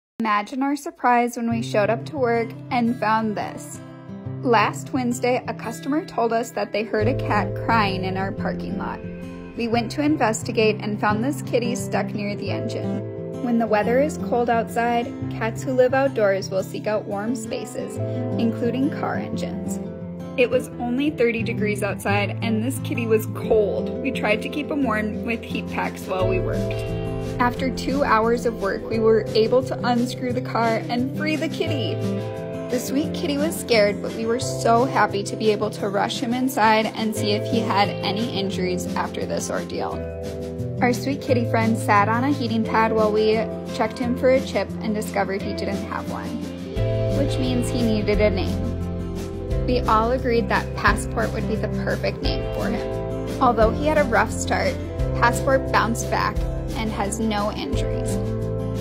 Cat rescued from a car
Our dedicated staff answered the call from a kitty trapped in the engine of a car.